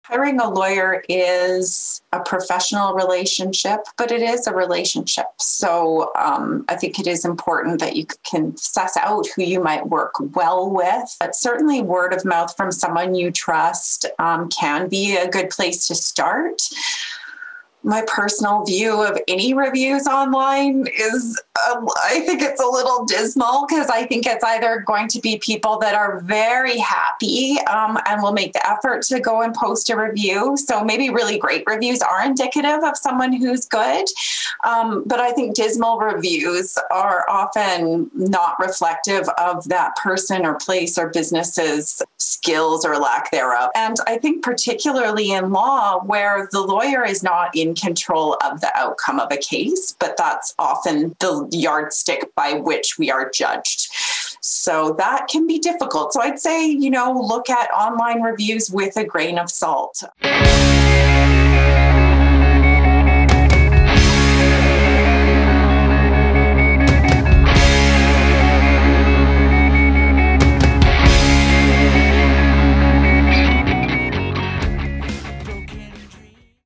Episode #3 - How to Shop for a Lawyer. Sneak Preview.
Join Access to Justice Podcast for Episode #3 - How to Shop for a Lawyer?

For the full episode visit a2jpodcast.com.